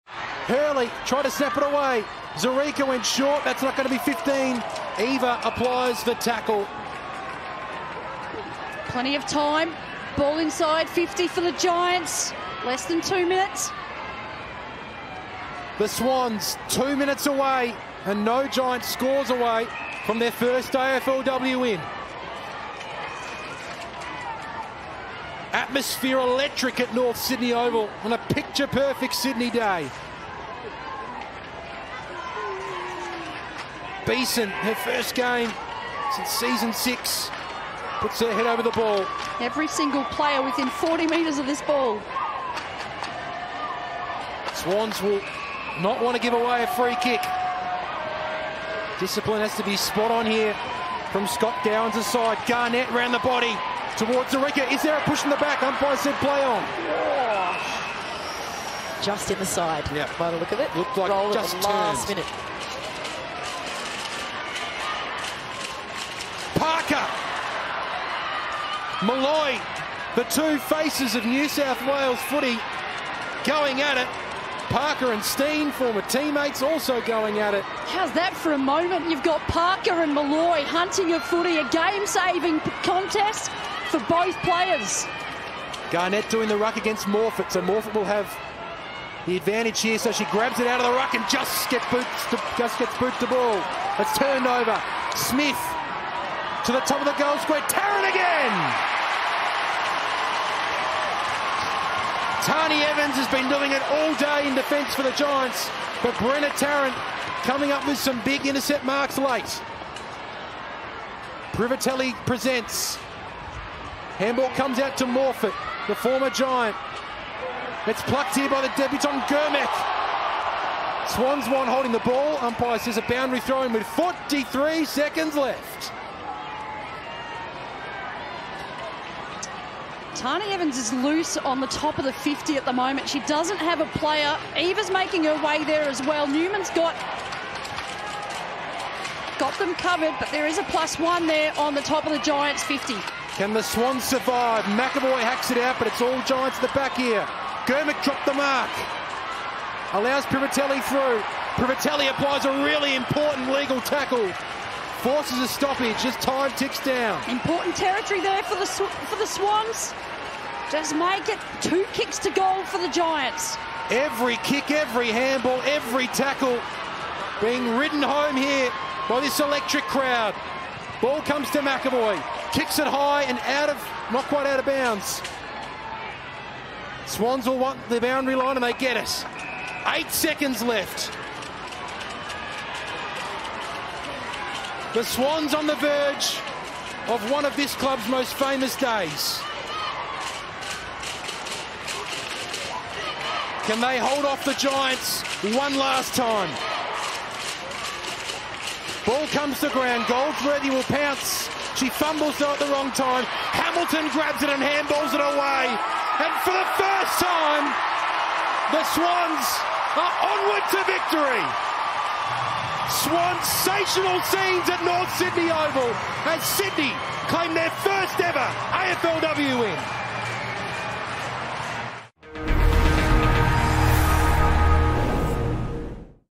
Last Two Minutes | Sydney v GWS Giants | Round 1, 2023 | AFLW
What a way to win secure your first ever AFLW win! 😍

Relive the last two minutes of the Sydney Derby.